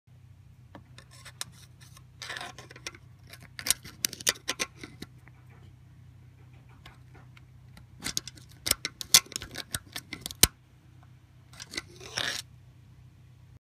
2006 F150 5.4L 3V timing chain
Replacing the VCT solenoid on this truck and inspecting the timing chain without removing the cam cover. I used the screwdriver to have more room for the camera. When I moved it by hand, I felt that I could probably easily push the chain off the cam phaser. Chain has no tension. Chain tensioners and guides require replacement. Engine was clattering and stalling at idle.